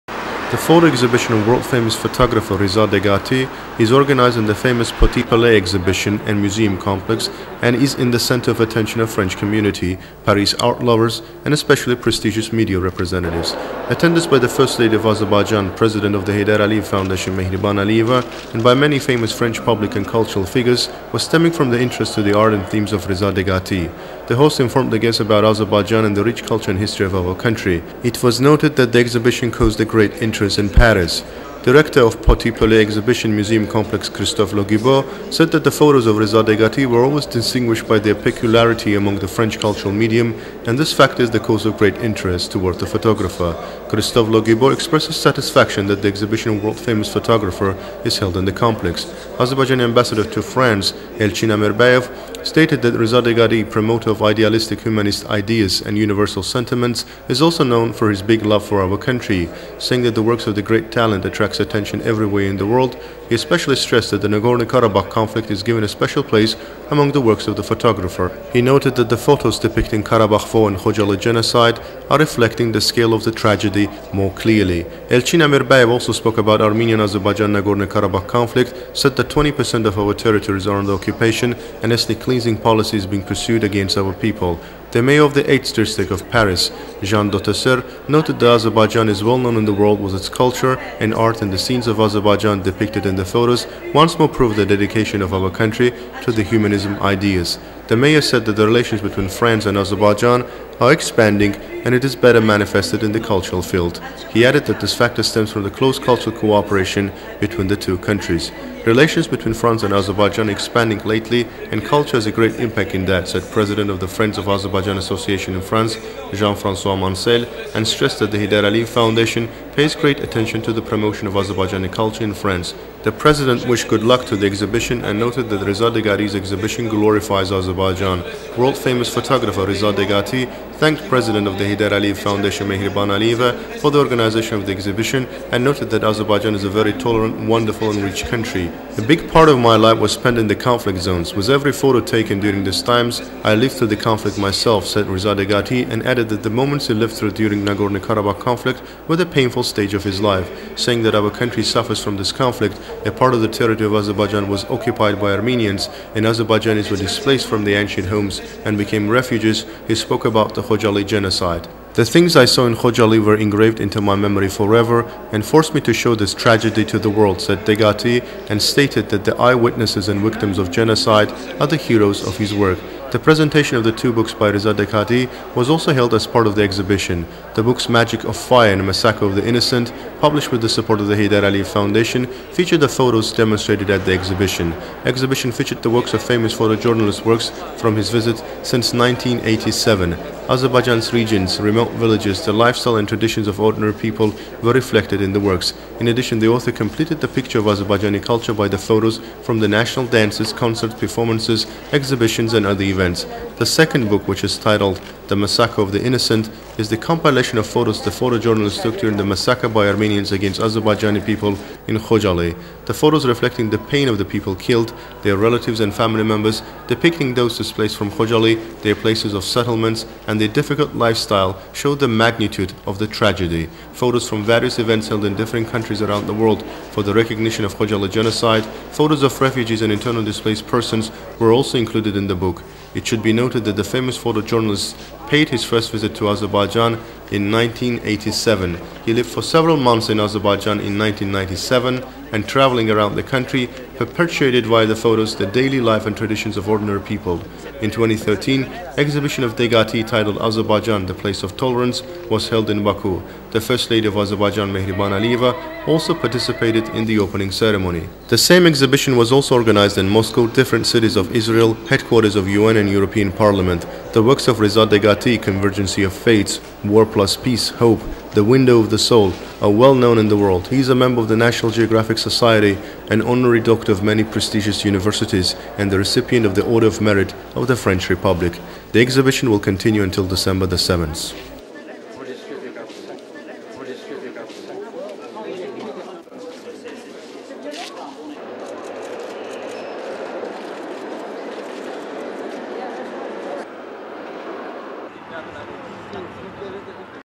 İnauguration of a photo-exhibition devoted to Azerbaijan takes place in Paris
Presentation of a photo-exhibition devoted to Azerbaijan and two books by world-renowned photographer Reza Deghati took place in Paris, November 28.

Azerbaijan’s First Lady, President of the Heydar Aliyev Foundation Mehriban Aliyeva and Foundation’s Vice-president Leyla Aliyeva attended the ceremony.

Organized at the Petit Palais Exhibition and Museum Complex of Paris, the exhibition was joined by well-known persons and persons of culture of France.

The host of the event informed the guests of Azerbaijan, its rich culture and history.

Director of the Petit Palais Exhibition and Museum Complex Christophe Leribault noted that photos by Reza Deghati are famous all over the world.

Ambassador of Azerbaijan to France Elchin Amirbeyov noted that, in the creativity of the photographer, the Daghlyg Garabagh conflict has its own place. The diplomat said photos by Reza Deghati that reflect the Garabagh War and Khojaly Genocide clearly picture the scale of the tragedy.

Mayor of the 8th District of Paris Mrs Jeanne d'Hauteserre  said Azerbaijan has become famous in the world for its culture and art. The Mayor underscored that relations between France and Azerbaijan were broadening, and this was more obvious in the field of culture.

Chairperson of the Friends of Azerbaijan Association in France Jean François Manselle too talked about the France-Azerbaijan relations. He noted that the Heydar Aliyev Foundation has been showing special attention to popularization of the Azerbaijani culture in France.

Then Reza Deghati thanked President of the Heydar Aliyev Foundation Mehriban Aliyeva for the organization of the exhibition. The photographer noted that Azerbaijan was a tolerant, beautiful and rich country: “Most of my life was spent in conflict zones. Every photo I took during the conflicts has made me live that conflict. What I experienced during the Daghlyg Garabagh conflict is also one of the bitter moments of my life.” Reza Deghati underscored that, as a result of the Daghlyg Garabagh conflict, part of the Azerbaijani territory was occupied by Armenia and Azerbaijanis have been displaced from their fatherlands. He talked about the Khojaly Genocide: “What I’ve seen in Khojaly have permanently remained in my memory and made me convey this tragedy to the whole world. Victims and martyrs of the Khojaly tragedy are main characters of my creativity”.

Two books by Reza Deghati were also presented within the framework of the exhibition. Published with the support of the Heydar Aliyev Foundation, the books “Elegance of Fire” and “Massacre of the Innocent” contain photos by Reza Deghati collected in the exhibition.

The book “Elegance of Fire” contains photos taken by Reza Deghati since 1987 during his trips to Azerbaijan. Regions, remote villages of Azerbaijan, lifestyle of common people, their traditions etc. have been reflected in the book. The author has, at the same time, created the view of Azerbaijani culture by photos taken during national dances, concerts, exhibitions and other events.

And in the book “Massacre of the Innocent”, photos taken by the photo-correspondent during the Khojaly genocide committed by Armenia against Azerbaijanis in 1992 have been collected. The book also contains photos from events held in various countries of the world aiming to get the Khojaly tragedy recognized in the world, and from a township for refugees and IDPs.